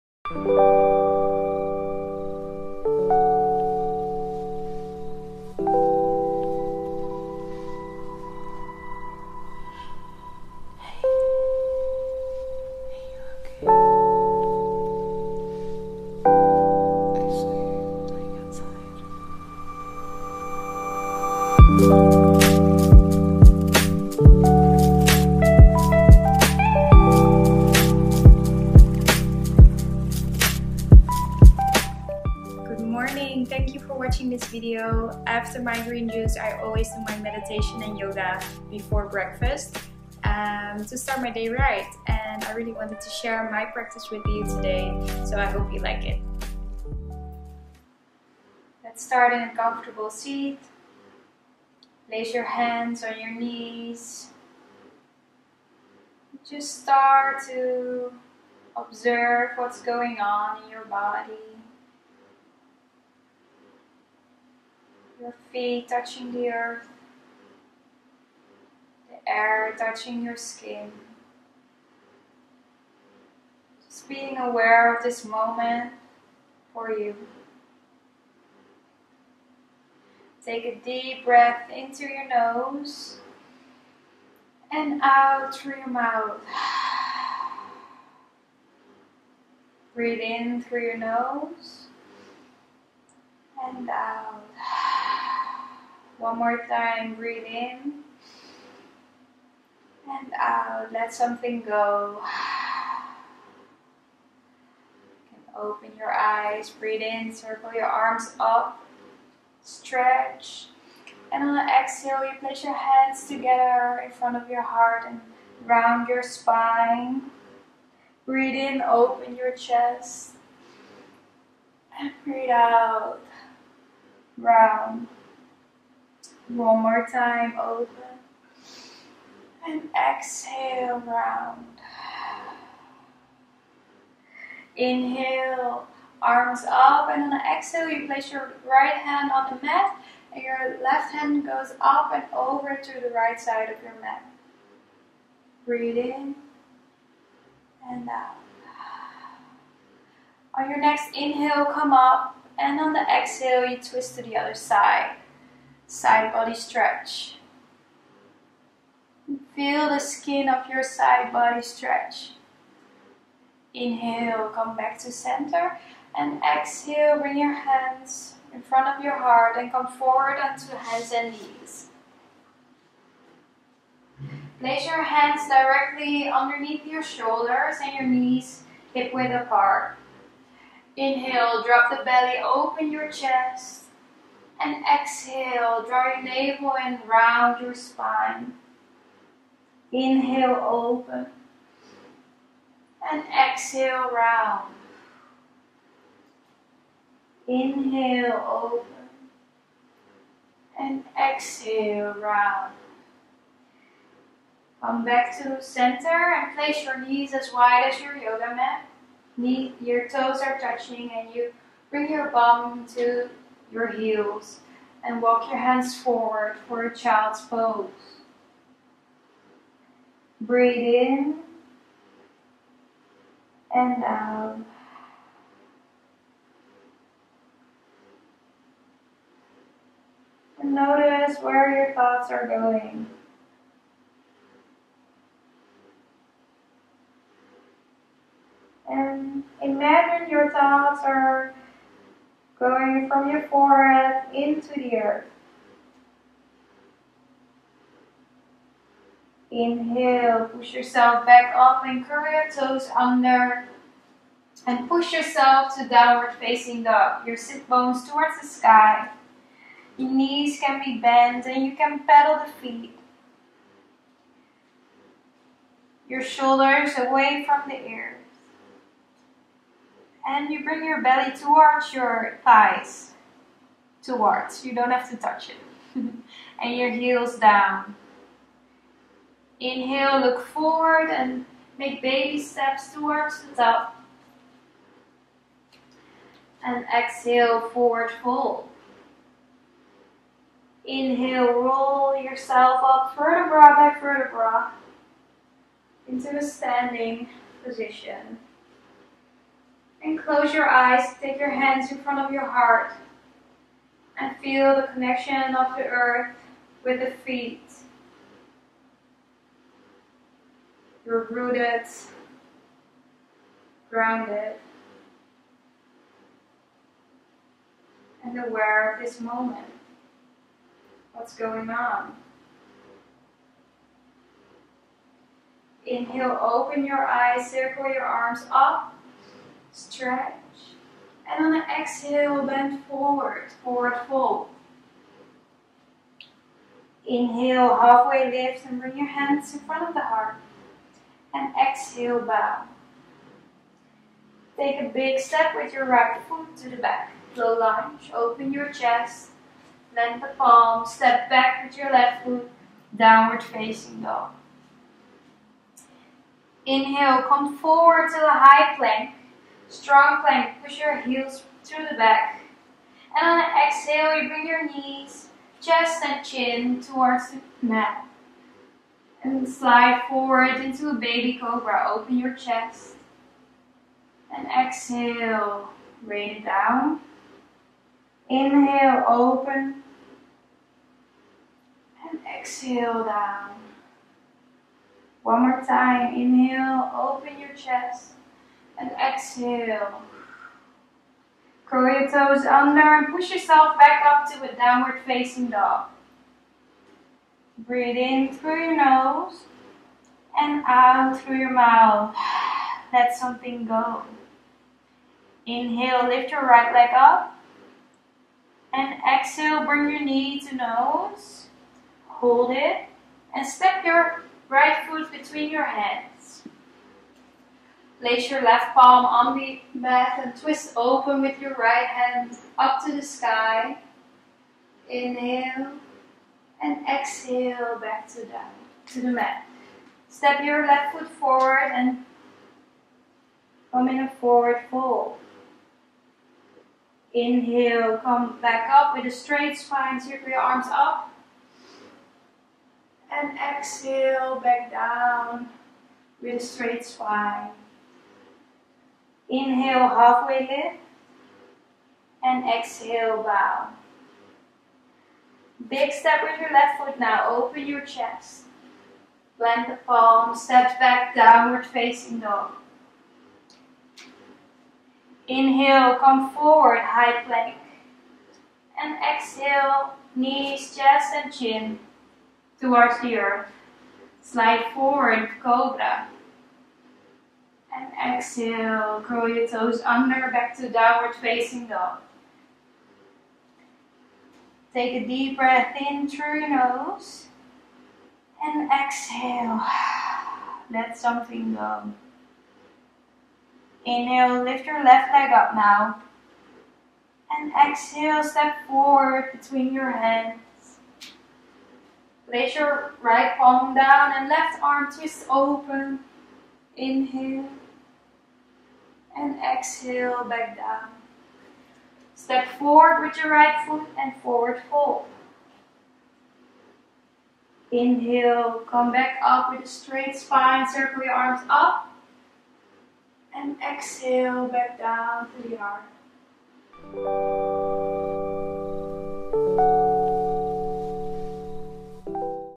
✧ START FRESH & ENERGIZED IN 10 MIN ✧ || + YOGA PRACTICE 🌟
Hi guys!

After a lot of requests on Instagram for a yoga video I finally made one! I hope you'll like it! With this short practice you will start your day fresh and energized!

What's important to me is to always keep in mind to feed your body with love, listen to your own body & your own needs. I hope to inspire you to make the best choices for your health from a place of love for yourself.

The Green Juice has a lot of health benefits because of all of the vitamins and minerals your body needs.

This video is in collaboration with Superfoodies EU.

Love,
Nathalie xx